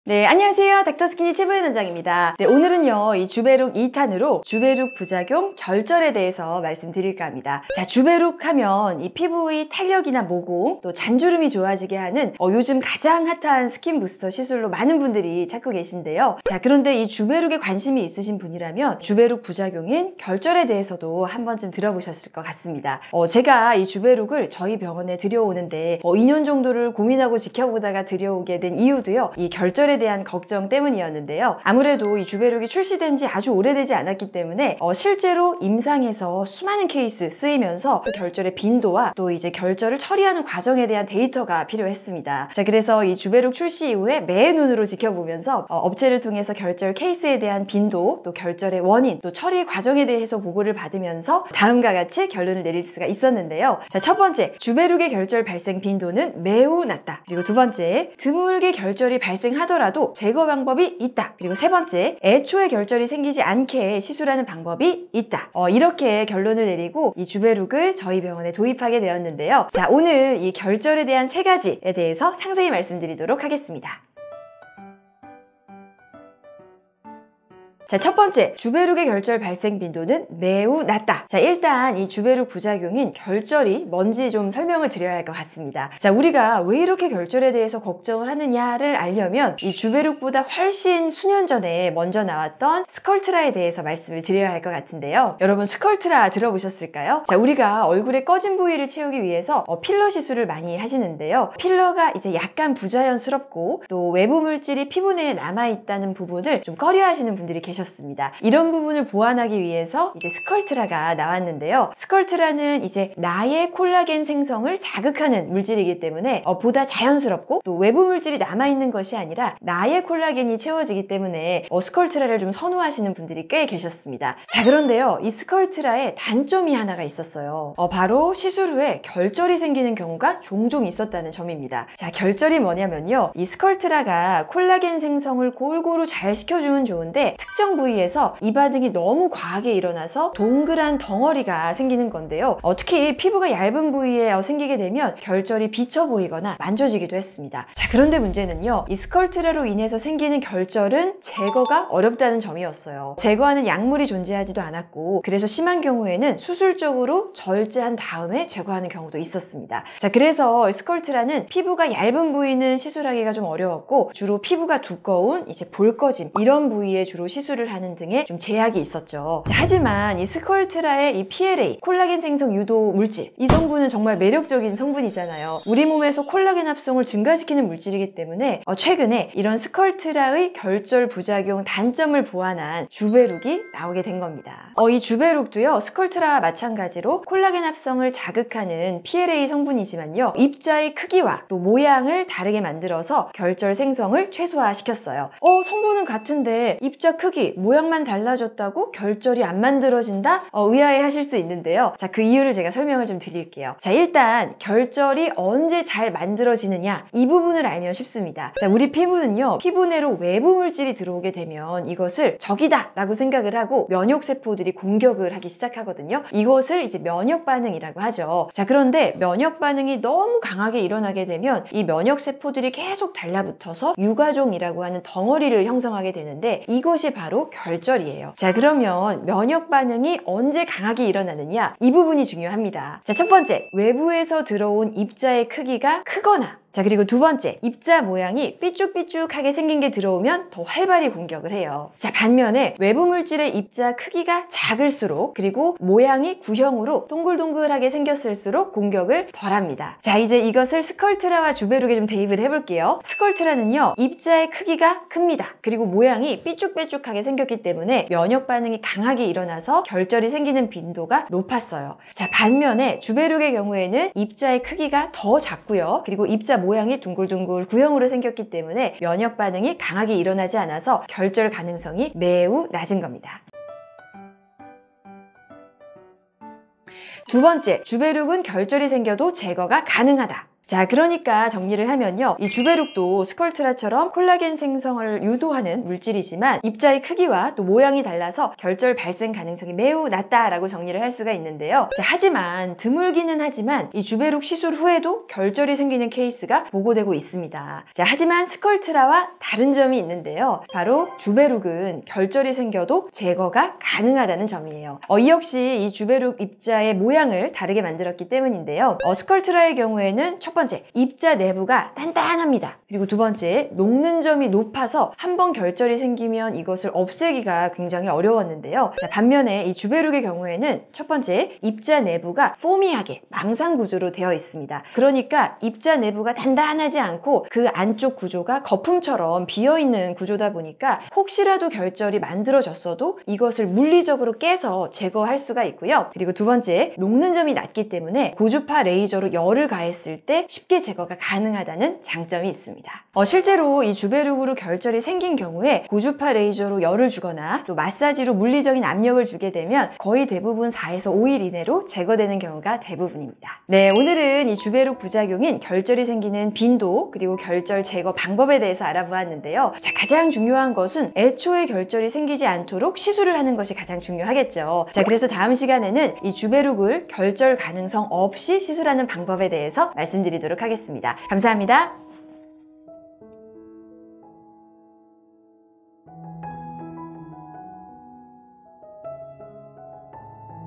[쥬베룩 2탄] 쥬베룩 부작용⚠️결절, 생길확률, 제거방법, 부작용 안생기는 시술❗ 시술 전 꼭 보세요❗❗
안녕하세요. 닥터스키니 최보윤 원장입니다.💃🏻

오늘은 쥬베룩 2탄으로 쥬베룩 부작용 안 생기게 시술하는 방법에 대해 말씀드릴까 합니다🍯

쥬베룩❗ 하면 피부 탄력, 모공, 잔주름이 좋아지게 하는 요즘 가장 핫한 스킨부스터 시술로, 많은 분이 찾고 계시는데요💬

그런데 쥬베룩에 관심이 있으신 분이라면, 쥬베룩 부작용인 ‘결절‘에 대해서도 한 번쯤 들어보셨을 것 같습니다❗

오늘은 쥬베룩 부작용인 결절에 대해서 상세히 말씀드리겠습니다💃🏻

모두들 예뻐질 준비 되셨나요?

질문사항은 댓글로 남겨주시면 답변 드리도록 하겠습니다 💜

몸과 마음이 건강해지는 그날까지 오늘도 파이팅하겠습니다💖